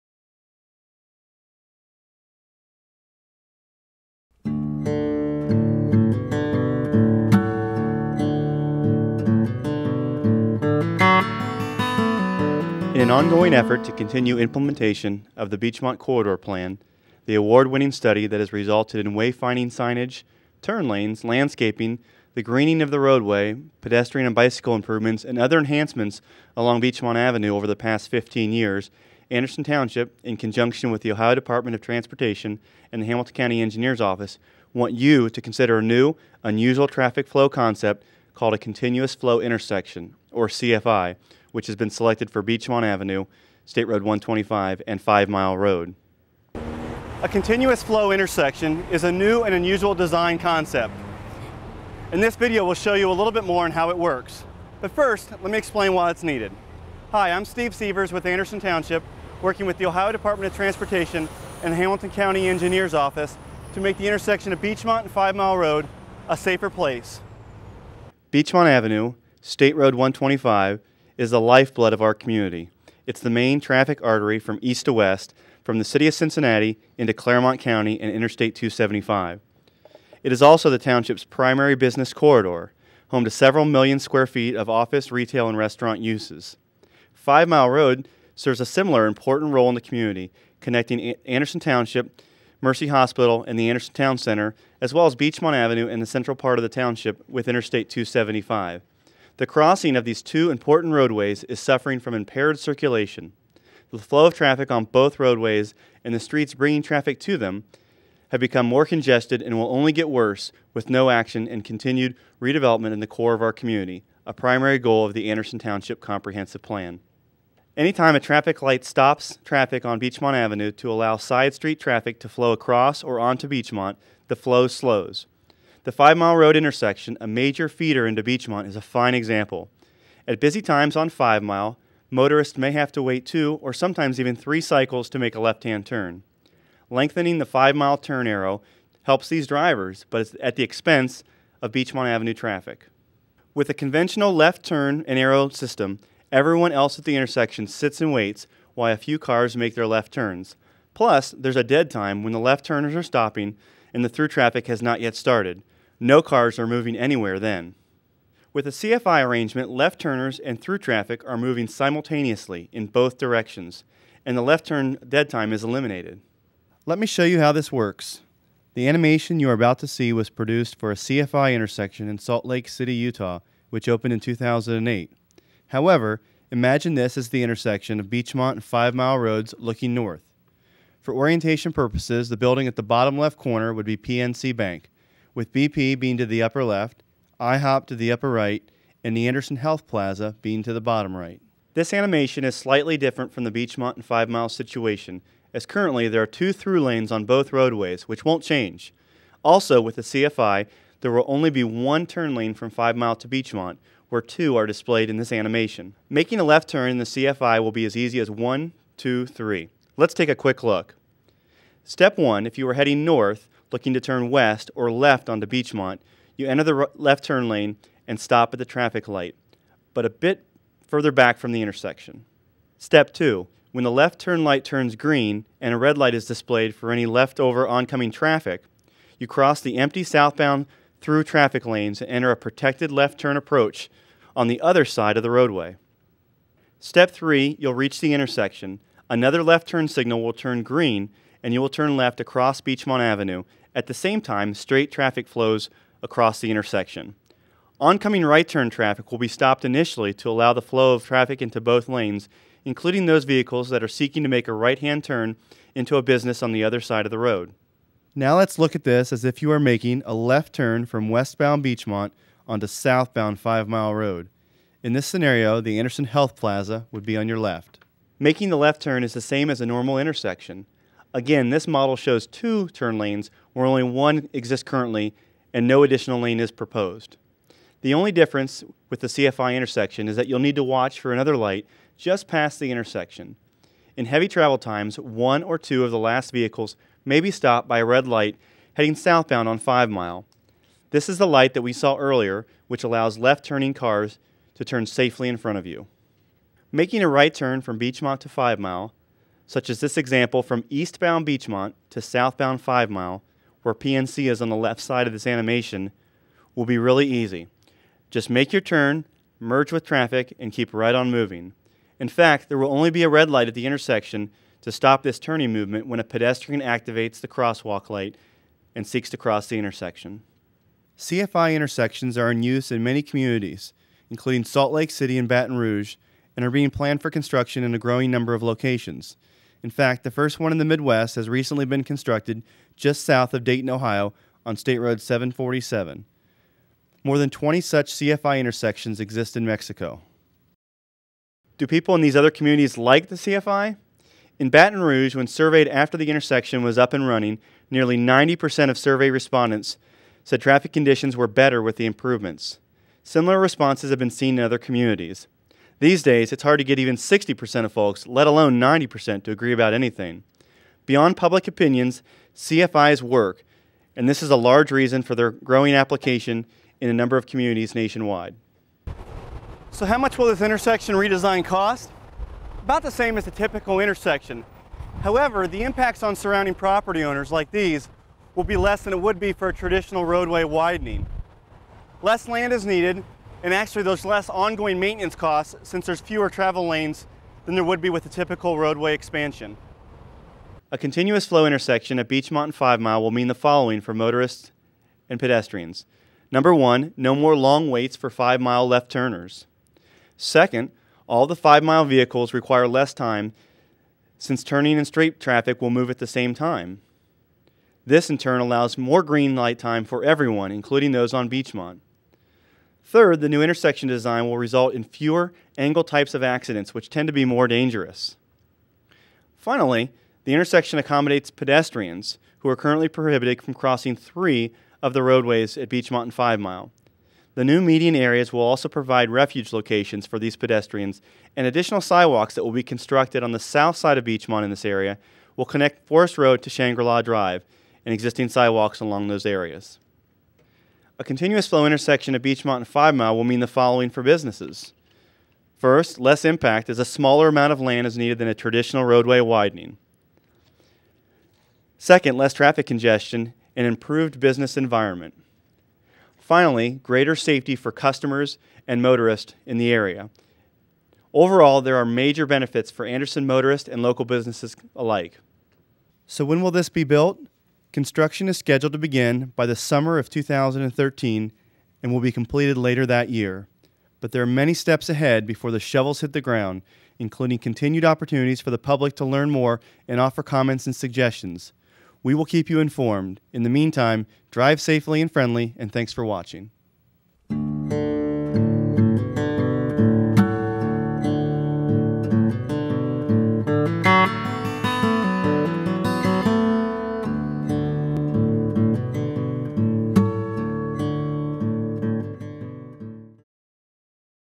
Anderson Township: Beechmont Ave & Five Mile Road Continuous Flow Intersection Informational Video
This video provides an overview of what a Continuous Flow Intersection (CFI) is and how it will work at the intersection of Beechmont Ave & Five Mile Road in Anderson Township, OH.  The video is narrated by Steve Sievers, Assistant Administrator for Operations, Anderson Township.